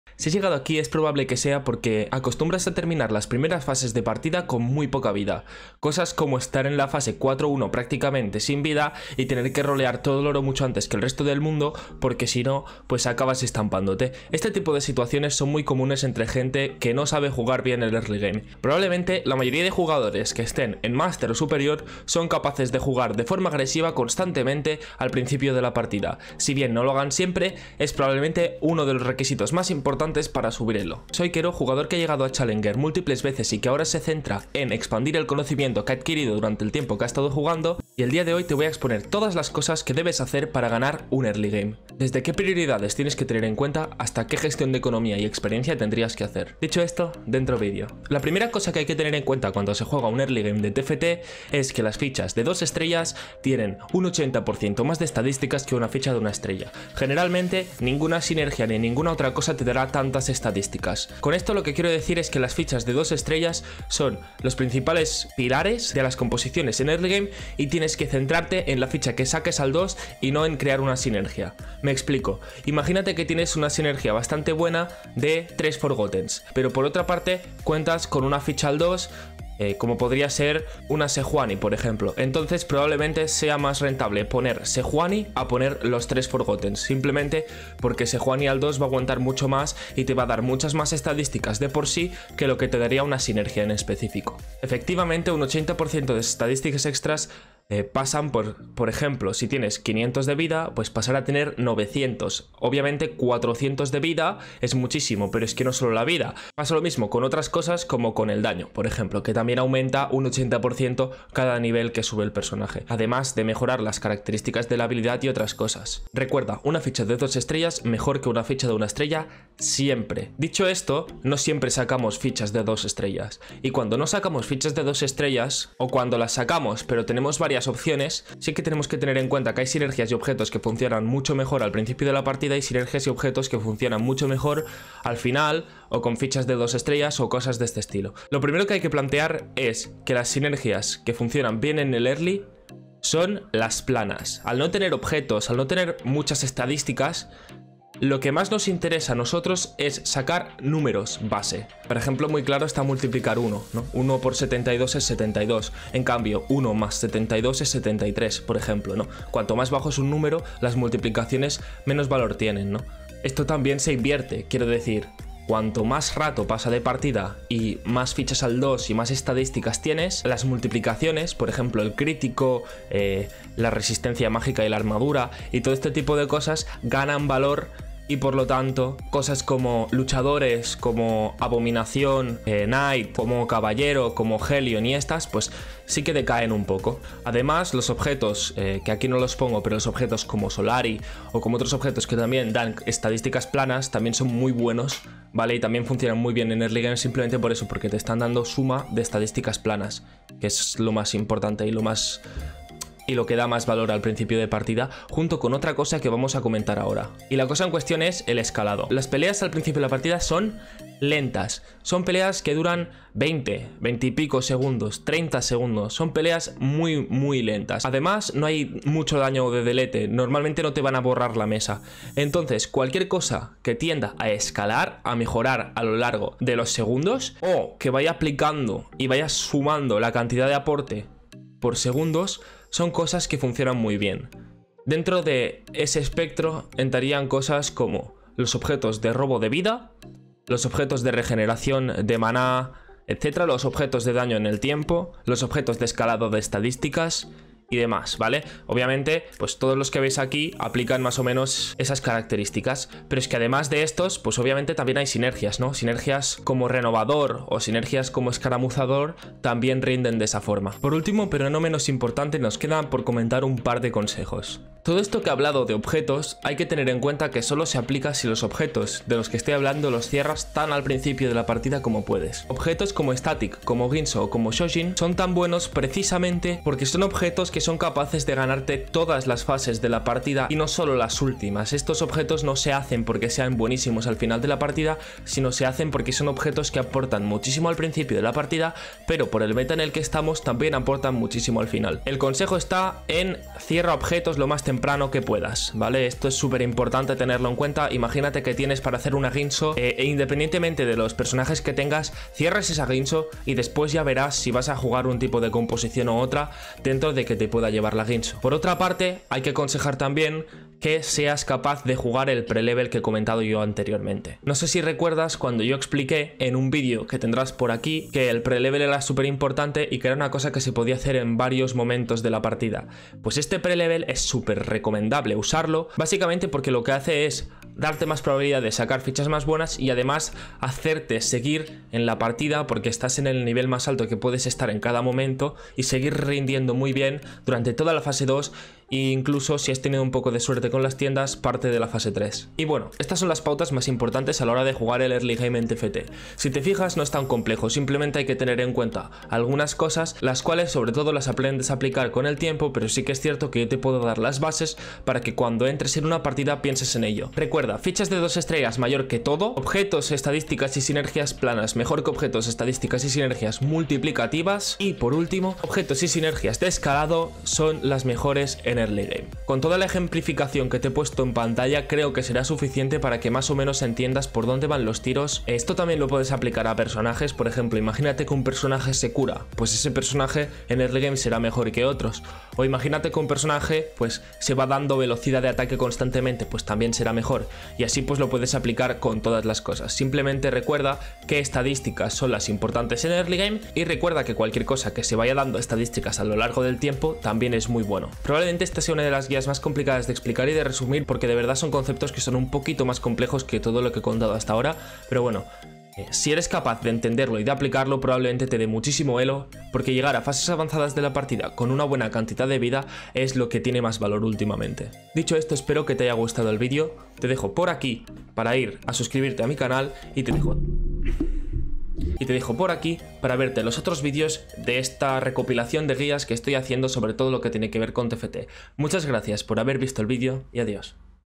💥GANA y SUBE ELO FÁCIL con ESTE TRUCO (FUNCIONA)✅ | PROYECTO: Diamante #05
SOY KERÖ💚 Y hoy te quiero enseñar un truco para subir elo fácilmente. Se trata de jugar bien el early. Este vídeo es parte de PROYECTO: Diamante💎.